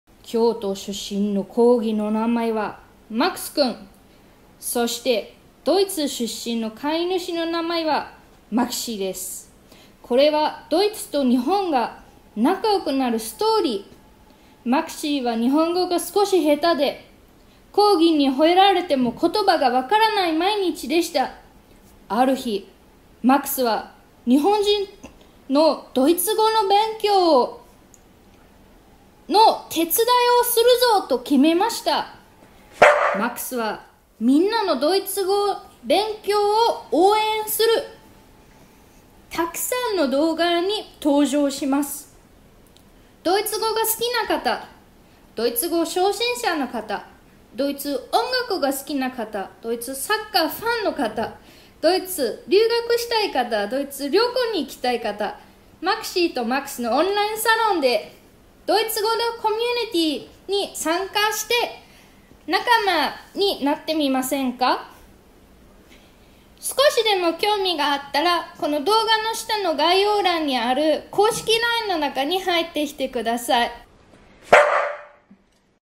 お知らせ‼️オンラインサロン！2021年4月1日公開予定🇩🇪
ドイツ語をもっとエンジョイできる
オンラインサロン！
2021年4月1日公開予定

京都出身のコーギーの名前はMAXくん。そして、ドイツ出身の飼い主の名前はMAXIEです。

これは「ドイツと日本が仲良くなる」ストーリー。

MAXIEは日本語が少し下手で、コーギーに吠えられても、言葉がわからない毎日でした。

ある日、MAXは「日本人のドイツ語の勉強のお手伝いをするぞ！」と決めました。

MAXはみなさんのドイツ語勉強を応援すべく、動画にたくさん登場します！

ドイツ語が好きな人、ドイツ語初心者の人、ドイツ音楽が好きな人、ドイツのサッカーファンの人、ドイツ留学したい人、ドイツ旅行に行きたい人……

MAXIEとMAXのオンラインサロンで、

ドイツ語のコミュニティに参加して仲間を作りませんか？

ひとつでも当てはまったら、このオンラインサロンはあなたの居場所になります。

サロン会員様　3つのメリット

オンラインサロン限定公開のドイツ語講座動画が見放題

オンラインサロンでは、YouTube非公開の数十本以上のドイツ語講座動画をたくさんご用意しています。

動画コンテンツは、ドイツ語初心者向けはもちろん、中級者・上級者向け、ドイツ語旅行で使えるフレーズなど随時更新していきます。

 2. ドイツ語に関する質問に動画でお答えします
ドイツ語の勉強で行き詰ったとき、勉強のモチベーションが下がってしまいますよね。

このオンラインサロンでは、MAXIEに質問することができ、サロン上の動画でお答えします。

他のサロンメンバーの質問に対する回答もこのオンラインサロンでは動画視聴できるので、学習の参考になるでしょう。

3. オンラインとオフラインでのイベント優先参加
オンラインサロン会員様はMAXIE主催イベントである少人数限定のオンライン飲み会、オンラインコーヒー会への優先参加権があります。ドイツ語の仲間作りにおすすめです。

MAXIEとMAXは日本とドイツの架け橋になるべく、みなさんのドイツ語学習を応援します。

このオンラインサロンに来れば、ドイツがもっと好きになる、ドイツ語学習のモチベーションが上がる、ドイツにもっと行きたくなる！

ドイツ大好き！ドイツ語大好き！な皆さんのご参加をお待ちしております。

オンラインサロン情報についても随時お知らせ予定です。ぜひクリックして、最新情報を入手してくださいね。